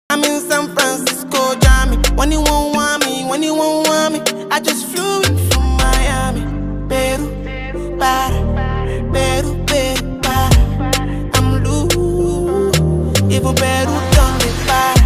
The REAL reason why Peru 🇵🇪 🦙 Traumlandschaften ⛰️ Stille, Natur peru lama plateau 🦙
Wunderschöne Landschaften mit tollen Tieren 🦙⛰️ Die Luft war ganz schön dünn zum atmen 😊